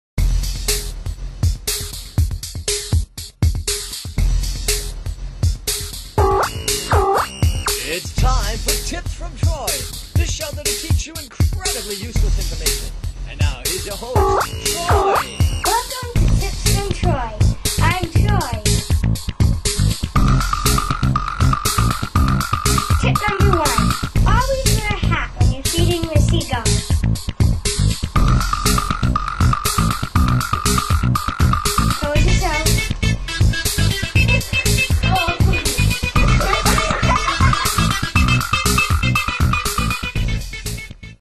Bird Poop!!!
A funny little video about a boy and bird poop.